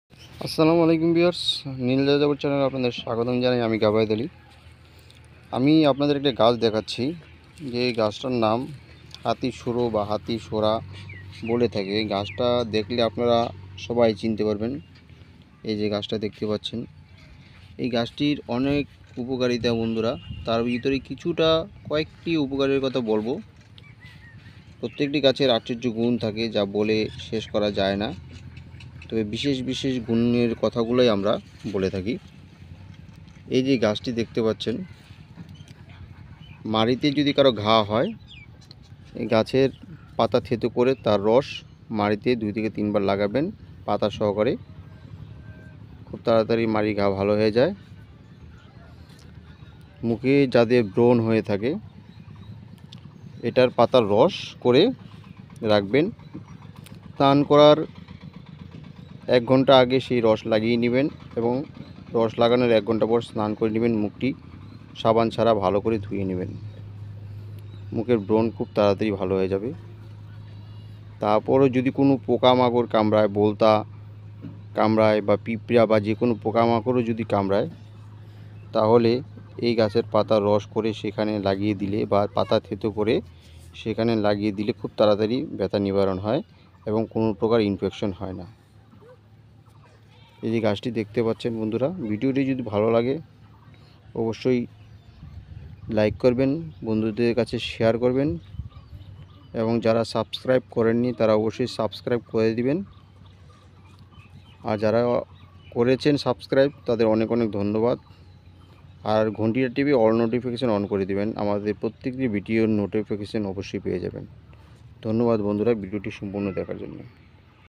হাতিসুরা গাছের উপকারিতা মুখের ব্রণ মাত্র 3 দিনে ঠিক করে
হাতিসুরা গাছের উপকারিতা মুখের ব্রণ মাত্র 3 দিন ঠিক করে মুখের ব্রণ মুখের কালো দাগ Gabaed Ali nil jajabar